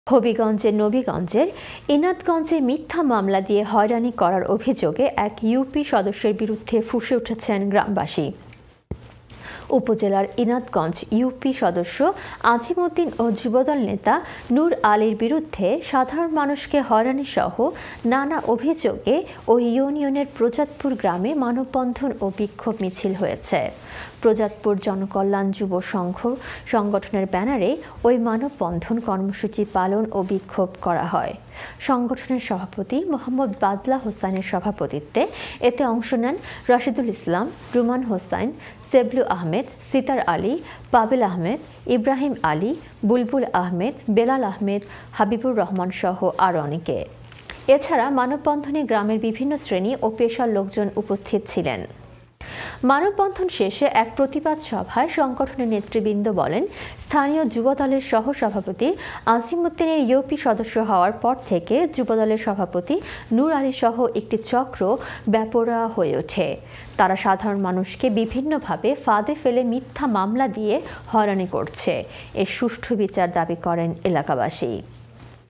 হবিগঞ্জের নবীগঞ্জের ইনাতগঞ্জে মিথ্যা মামলা দিয়ে হয়রানীকরার অভিযোগে এক ইউপি সদস্যের বিরুদ্ধে ফুঁসে
হবিগঞ্জের নবীগঞ্জের ইনাতগঞ্জে মিথ্যা মামলা দিয়ে হয়রানী
করার অভিযোগে এক ইউপি সদস্যের বিরুদ্ধে ফুঁসে উঠেছেন গ্রামবাসী। উপজেলার
ইনাতগঞ্জ ইউপি সদস্য আজিম উদ্দিন ও যুবদল নেতা নূর আলীর বিরুদ্ধে সাধারণ
মানুষকে হয়রানীসহ নানা অভিযোগে ওই ইউনিয়নের প্রজাতপুর গ্রামে মানববন্ধন
ও বিক্ষোভ মিছিল হয়েছে। প্রজাতপুর জনকল্যাণ যুব সংঘ সংগঠনের ব্যানারে ওই
মানববন্ধন কর্মসূচি পালন ও বিক্ষোভ করা হয়। সংগঠনের সভাপতি মোহাম্মদ
বাবলা হোসাইনের সভাপতিত্বে এতে অংশ নেন রাশিদুল ইসলাম, রুমান হুসাইন, সেবলু
আহামেদ, সিতার আলী, পাবেল আহমেদ, ইব্রাহীম আলী, বুলবুল আহমেদ, বেলাল
আহমেদ, হাবিবুর রহমানসহ আরো অনেকে। এছাড়া মানববন্ধনে গ্রামের বিভিন্ন
শ্রেণী ও পেশার লোকজন উপস্থিত ছিলেন। মানববন্ধন শেষে এক প্রতিবাদ সভায়
সংগঠনের নেতৃবৃন্দ বলেন-,স্থানীয় যুবদলের সহ সভাপতি আজিম উদ্দিন ইউপি
সদস্য হওয়ার পর থেকে যুবদলের সভাপতি নুর আলীসহ একটি চক্র বেপরোয়া হয়ে
উঠে। তারা সাধারণ মানুষকে বিভিন্নভাবে ফাঁদে পেলে মিথ্যা মামলা দিয়ে হয়রানী
করছে। এর সুষ্ঠু বিচার দাবী করেন এলাকাবাসী।
iON TV
SKY 782